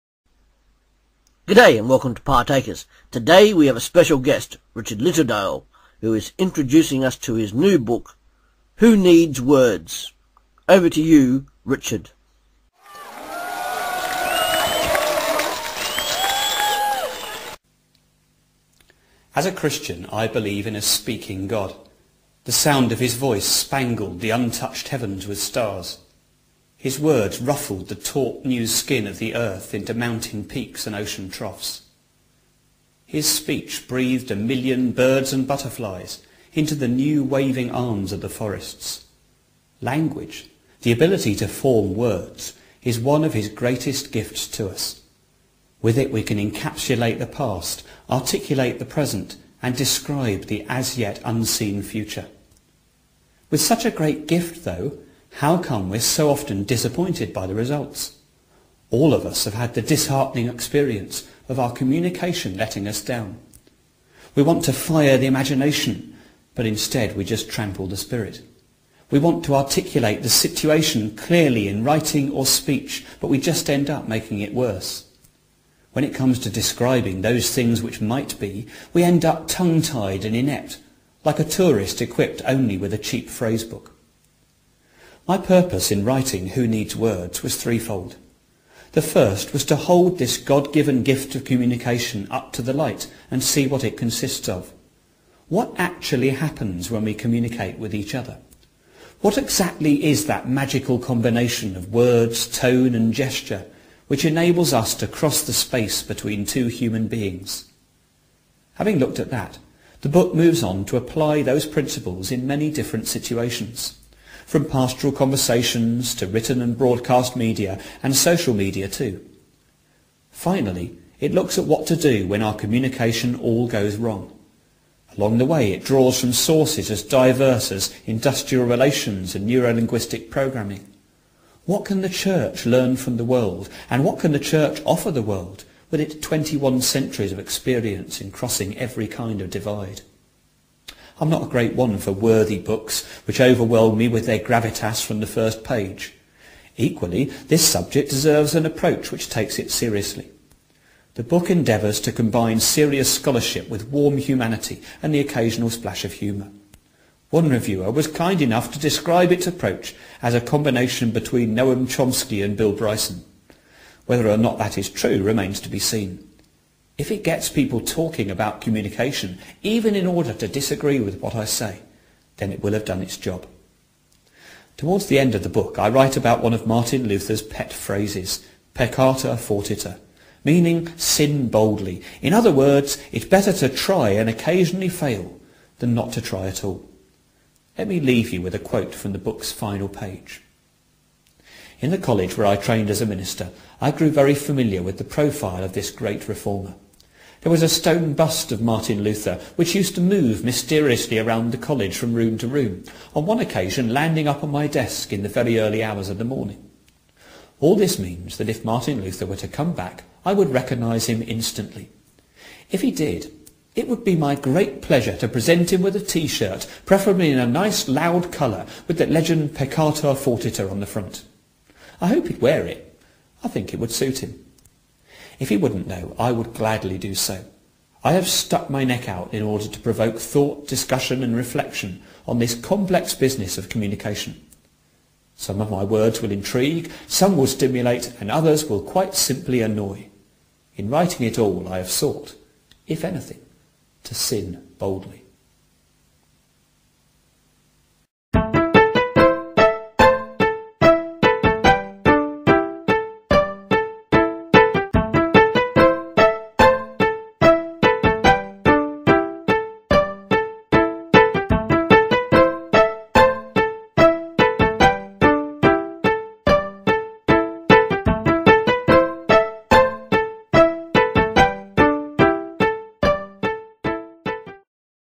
rl_wnw.wmv
G'day and welcome to Partakers! Today we have a special guest, Richard Littledale, who is introducing us to his new book, "Who Needs Words?"